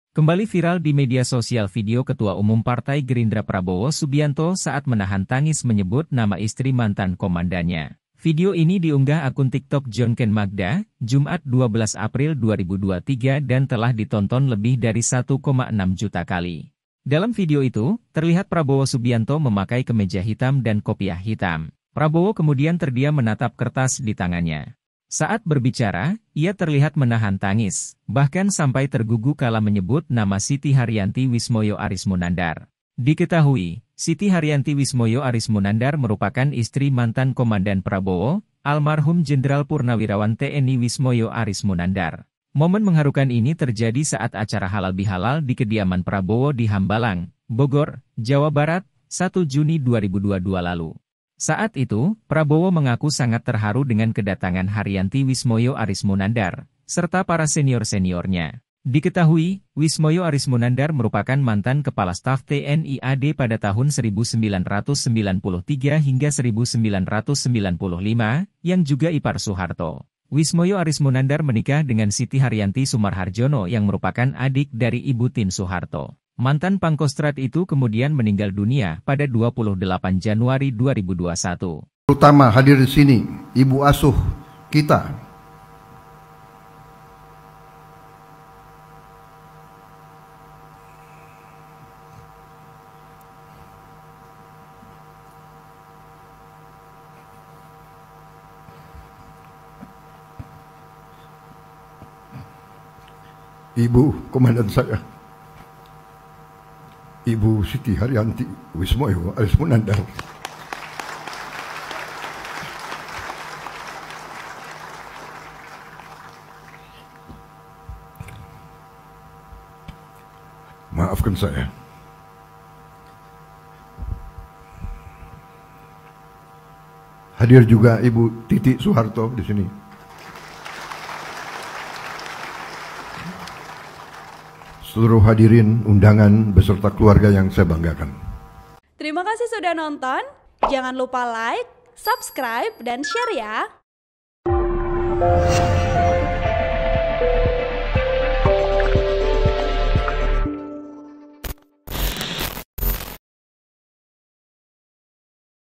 Kembali Viral Video Prabowo Terdiam Tahan Tangis Sebut Nama Istri Wismoyo Arismunandar: Maafkan Saya
Dalam video itu, terlihat Prabowo Subianto memakai kemeja hitam dan kopiah hitam.

Prabowo kemudian terdiam menatap kertas di tangannya.

Saat berbicara, ia terlihat menahan tangis, bahkan sampai tergugu kala menyebut nama Siti Hardjanti Wismoyo Arismunandar.

Diketahui, Siti Hardjanti Wismoyo Arismunandar merupakan istri mantan komandan Prabowo, almarhum Jenderal (Purn) TNI Wismoyo Arismunandar.

"Terutama hadir di sini, ibu asuh kita, ibu komandan saya, Ibu Siti Hardjanti Wismoyo Arismunandar, maafkan saya," ujar Prabowo.

"Hadir juga ibu Titiek Soeharto di sini," sambungnya.

Setelah ditelusuri TribunWow, rupanya momen mengharukan ini terjadi saat acara halalbihalal di kediaman Prabowo di Hambalang, Bogor, Jawa Barat, 1 Juni 2022 lalu.

Saat itu, Prabowo mengaku sangat terharu dengan kedatangan Siti Hardjanti Wismoyo Arismunandar, serta para senior-seniornya.

Diketahui, Wismoyo Arismunandar merupakan mantan Kepala Staf TNI AD (KSAD) pada tahun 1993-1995, yang juga ipar Soeharto.

Wismoyo Arismunandar menikah dengan Siti Hardjanti Soemaoerharjono yang merupakan adik dari Ibu Tien Soeharto.

Mantan Pangkostrad itu kemudian meninggal dunia pada 28 Januari 2021, saat menjalani perawatan di RS Pondok Indah, Jakarta di usia ke-80.